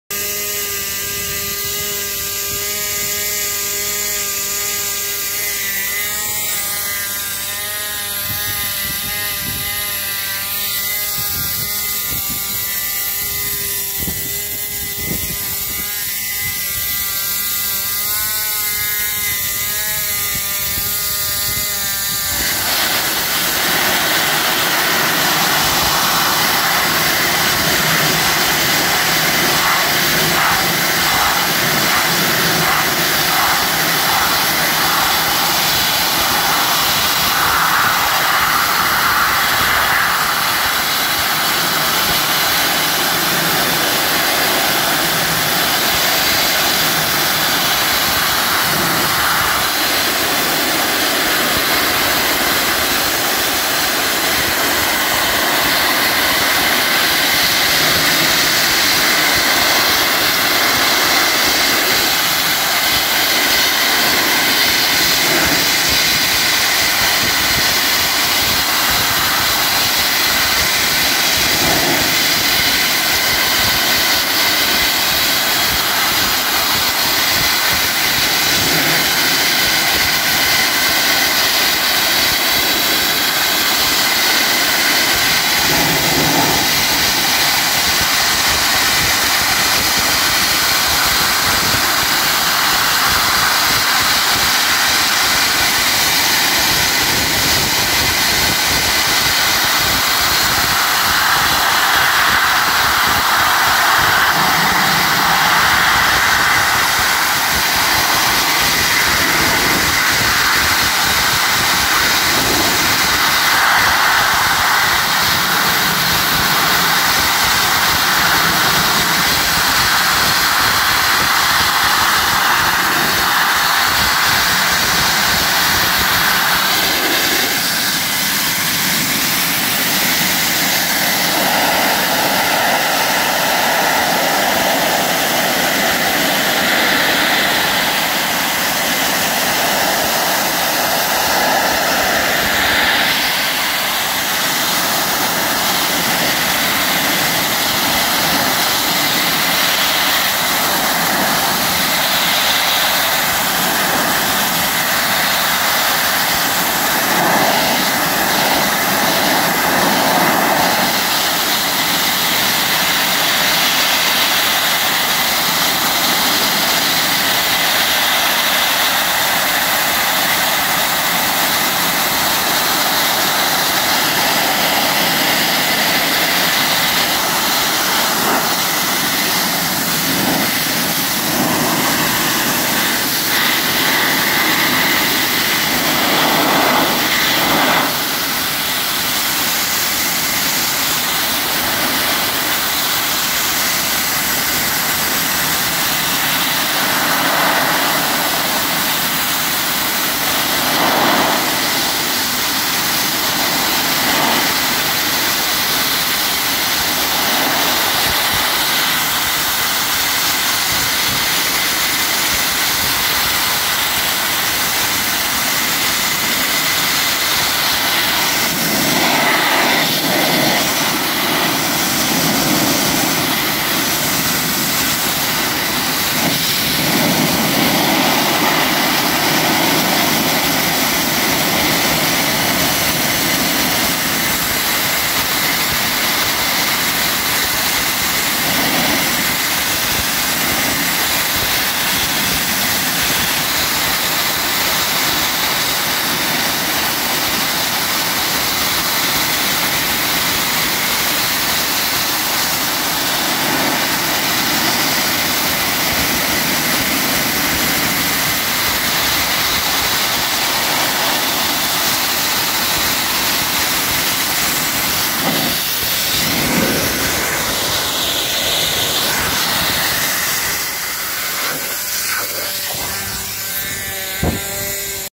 Dustless Blasting Rust Removal from Steel
Central Jersey Dustless Blasting can remove rust from steel without wearing the steel thin avoiding concern about the structural integrity, while preparing it for a fresh coat of paint or treatment to prevent future rusting.